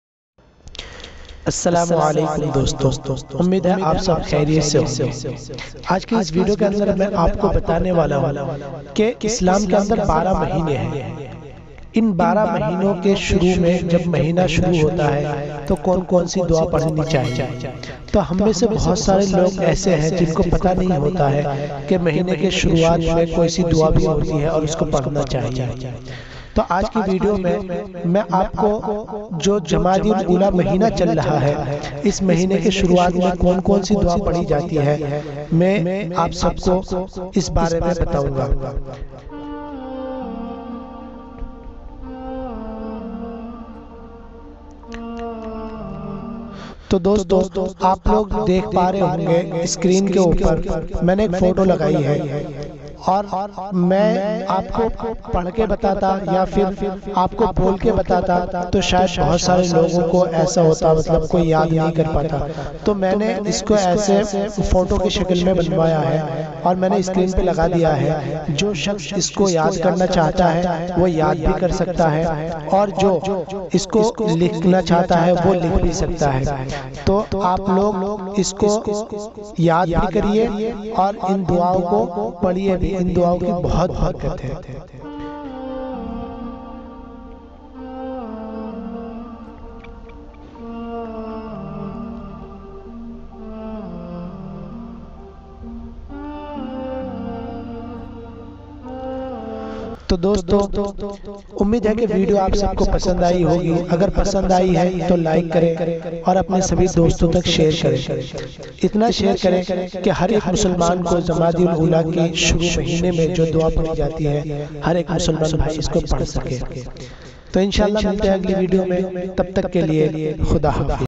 Mahine Ke Shuru Me Padhne Wali Dua ! jamadi UL ula Ke Shuru Me Konsi Dua Padhe !Maulana Saidul Islam
is video me ap janenge Ke Mahine Ke Shuru Me Padhne Wali Dua ! jamadi UL ula Ke Shuru Me Konsi Dua Padhe !Maulana Saidul Islam